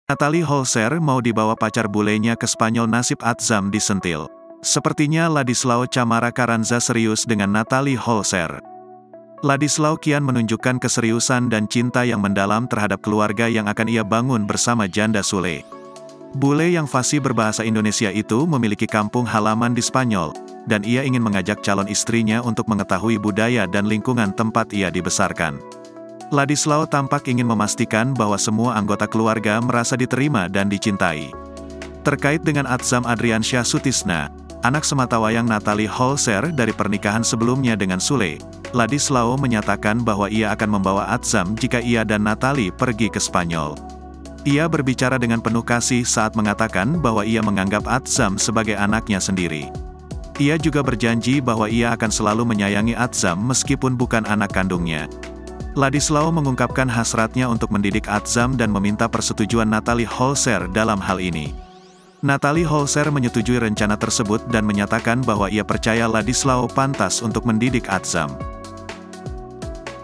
Nathalie Holscher mau dibawa pacar bulenya ke spanyol nasib Adzam disentil | Tribun Lampung
Nathalie Holscher mau dibawa pacar bulenya ke spanyol nasib Adzam disentil